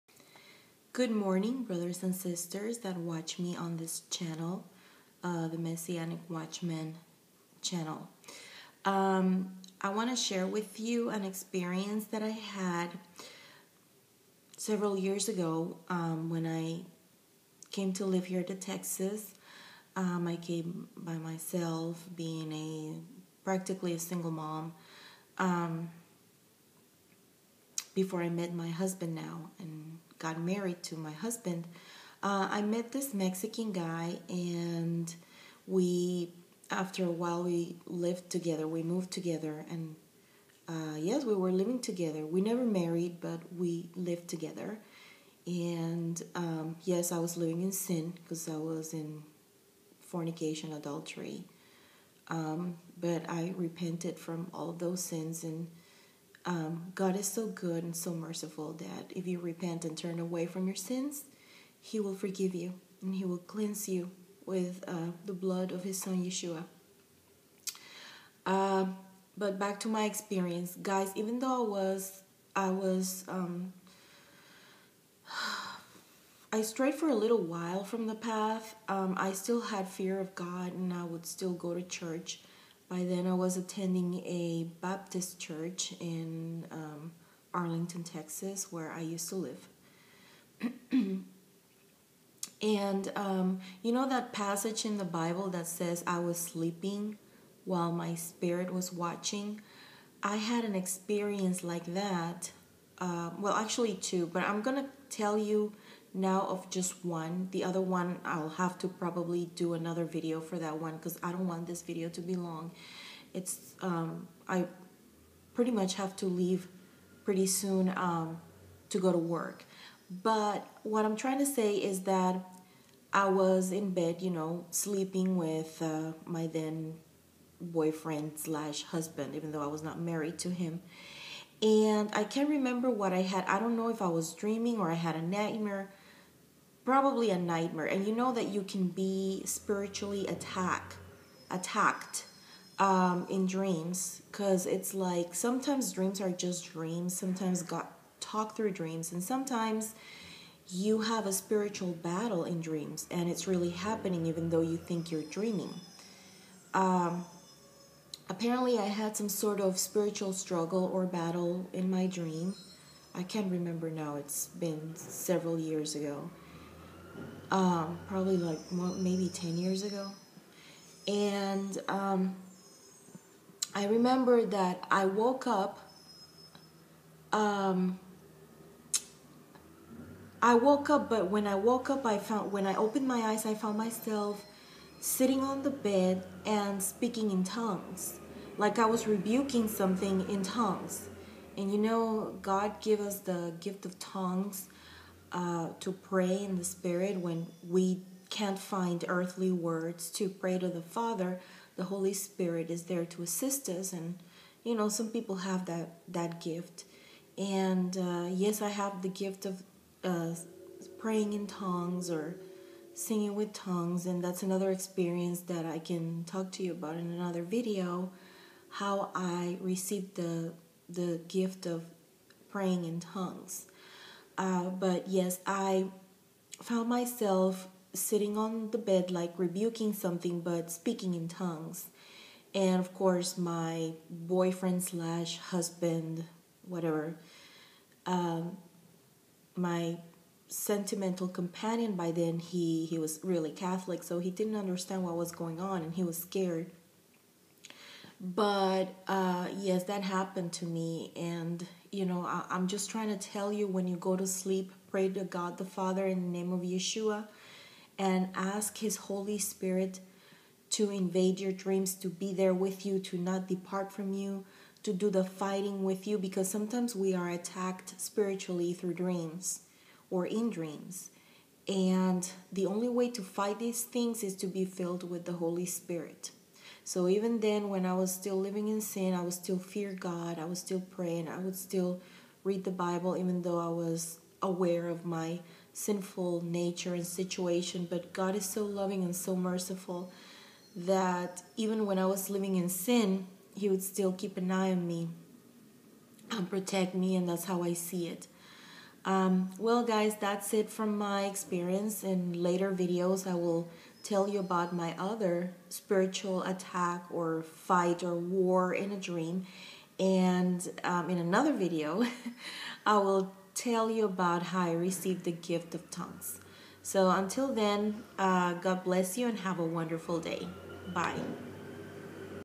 Being spiritually attacked in dreams
There's a Bible verse that says:  "I was sleeping while my spirit was watching."  That happened to me in this experience.  Thank God for His love, mercy, and care; but most of all, for His Holy Spirit.